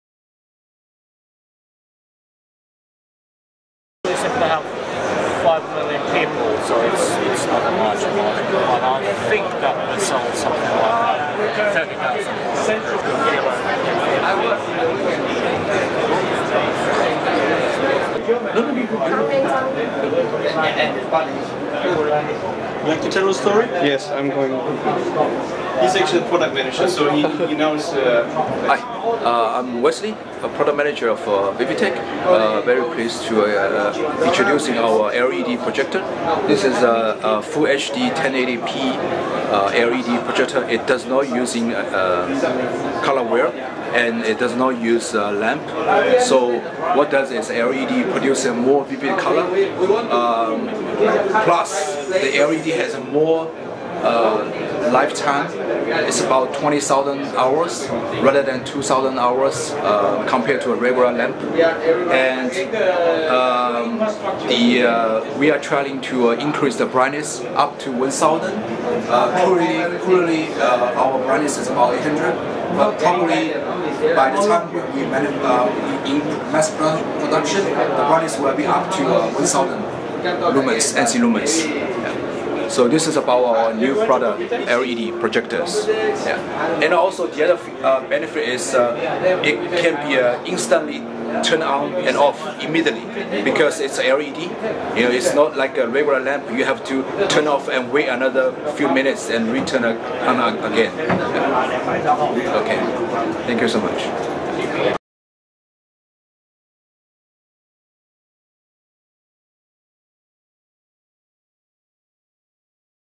LED Heimkinoprojektor von Vivitek - Modell 7500
Erster Voll HDTV Heimkinoprojektor mit LED und DLP Technik. 20.000 Stunden LED Lebensdauer, ca. 700 Ansi Lumen Hell und volle HDTV Auflösung. Preis ca. 20.000 Euro. Präsentiert auf der ISE 2009 in Amsterdam.
Wir beraten Sie gerne - verfügbar ab Sommer 2009